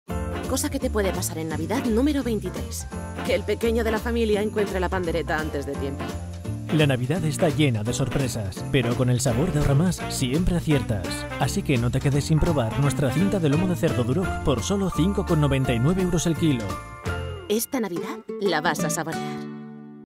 Esta Navidad la vas a saborear - Cinta de lomo de cerdo Duroc
Cosa que te puede pasar en Navidad #23: ¡que el pequeño de la familia encuentre la pandereta antes de tiempo!

La Navidad está llena de sorpresas, pero con el sabor de Ahorramas siempre aciertas. No te quedes sin probar nuestra cinta de lomo de cerdo Duroc por solo 5,99€/kg.

Esta Navidad la vas a saborear.